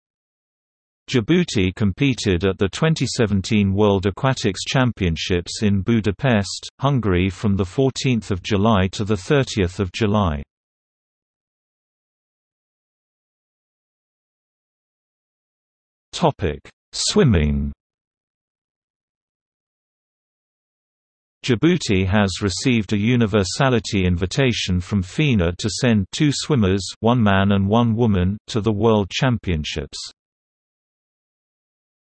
Djibouti at the 2017 World Aquatics Championships | Wikipedia audio article
00:00:12 Swimming

- Socrates

SUMMARY
Djibouti competed at the 2017 World Aquatics Championships in Budapest, Hungary from 14 July to 30 July.